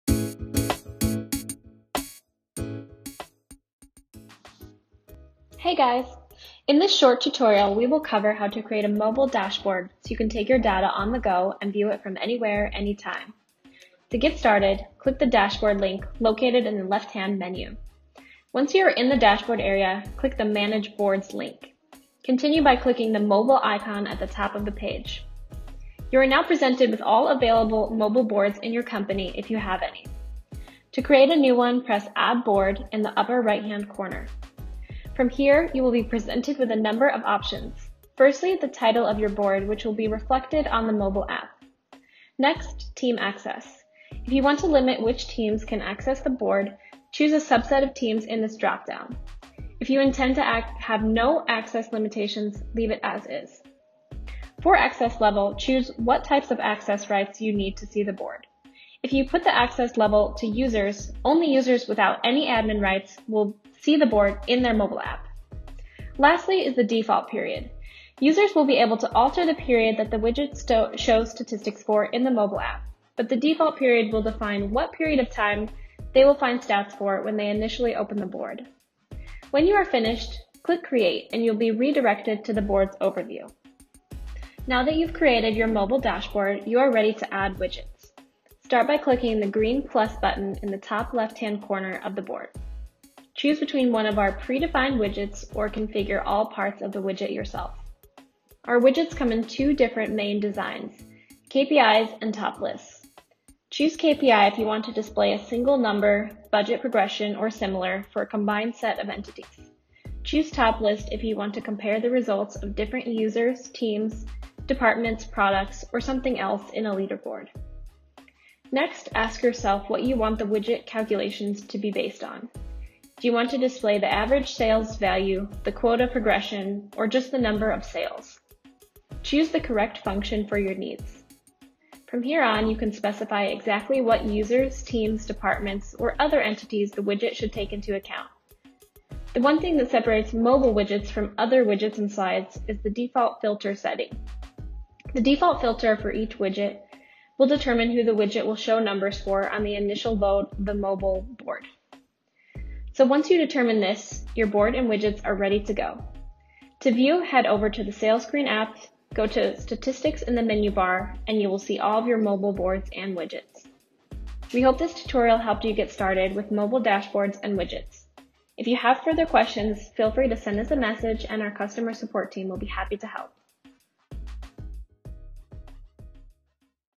SalesScreen Tutorials: Mobile Dashboards and Widgets
In this short tutorial, we will cover how to create a mobile dashboard, so you can take your data on-the-go and view it from anywhere, anytime.